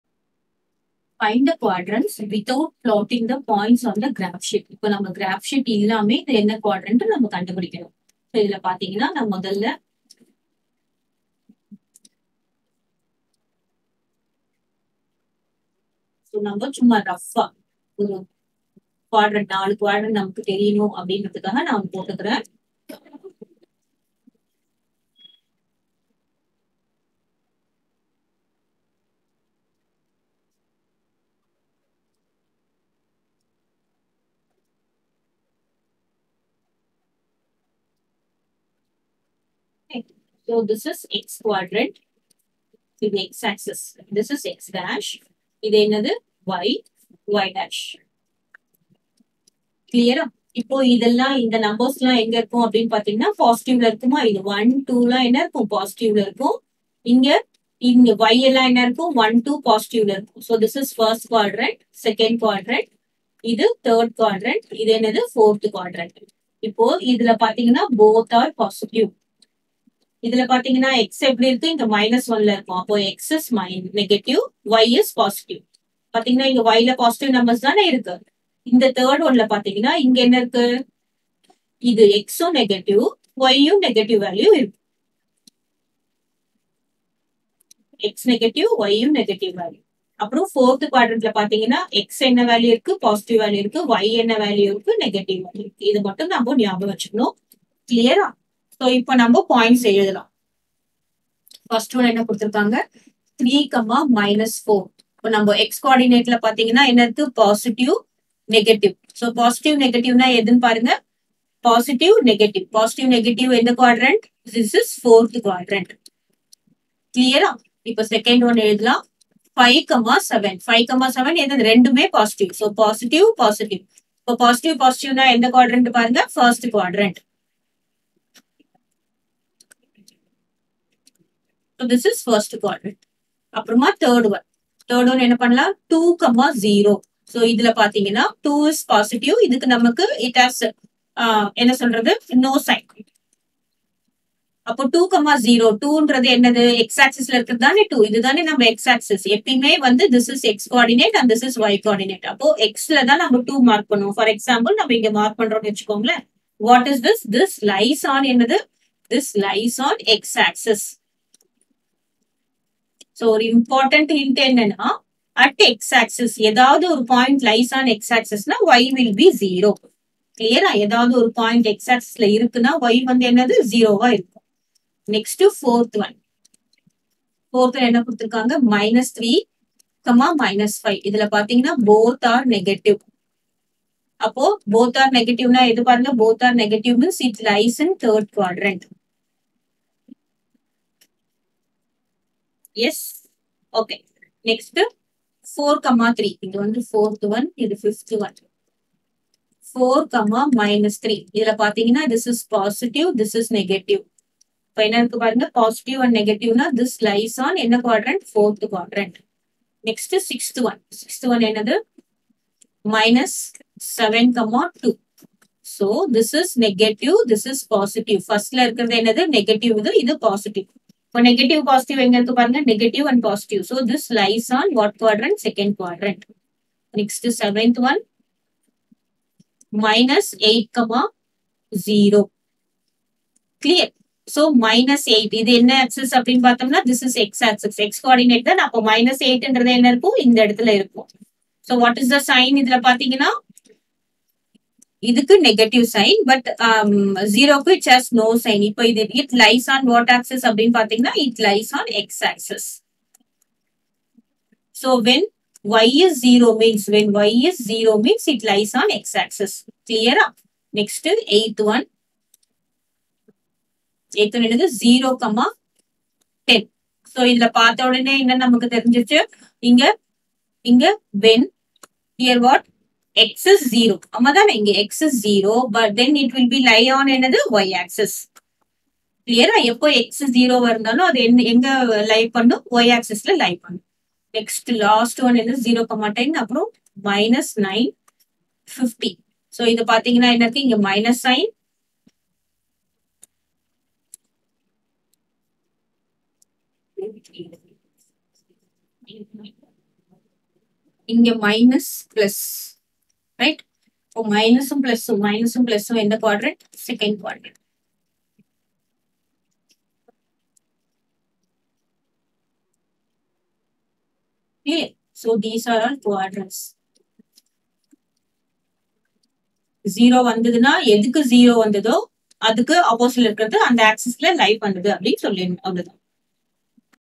Class 8 TN Maths 3 ALGEBRA Exercise 3.8 3 Find the quadrants without plotting the points
Exercise 3.8
3 Find the quadrants without plotting the points on a graph sheet.